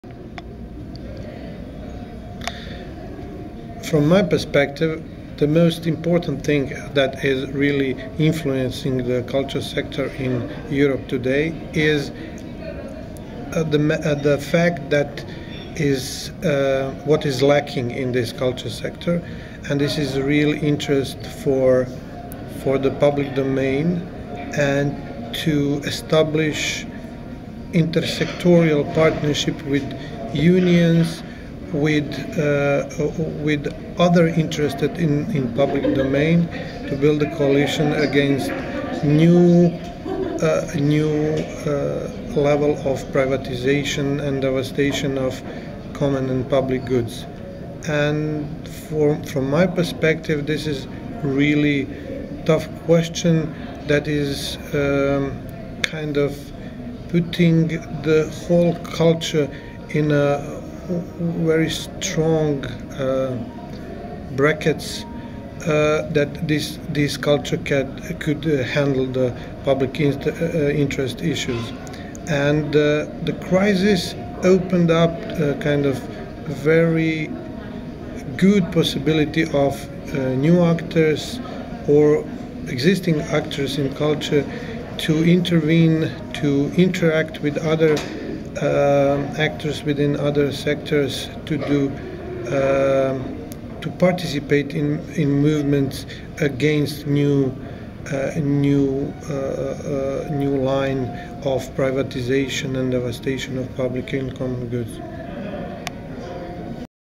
Snapshot Interview with Teo Celakowski
Teo Celakowsk, is connected to multimedia institute - mama  in Croatia and discusses what it's like trying to protect public spaces in a country that is "modernising" at a rapid rate.

In this clip he answers the question - what is currently the biggest impact on the European cultural scene?

He speaks with Nicola Mullenger from the European Cultural Foundation in Kiev whilst taking part in the European Neighbourhood programme cultural development workshop run by Phil Wood and in conjunction with the Centre for Cultural Management in Lviv.